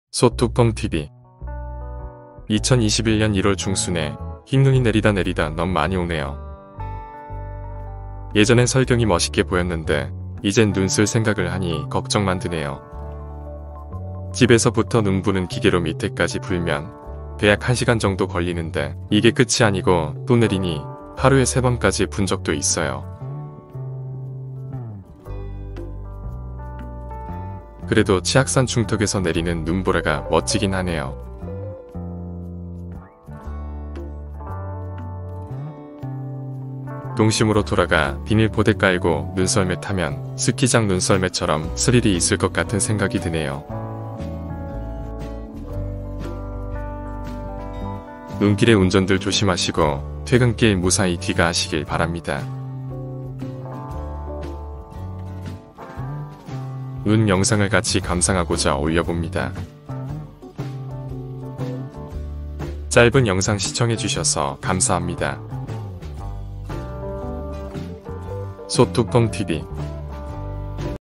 2021년1월 치악산 중턱에서 눈내리는 영상.
1월에 내리는 설경이 넘 멋있내요.
눈내리는걸 보내 소주가 한잔 땡겨지는데요^^ㅎㅎㅎ
금새 사방이 흰세상으로 변해서 마치 영화속의 한장을 보고 있는것 같내요.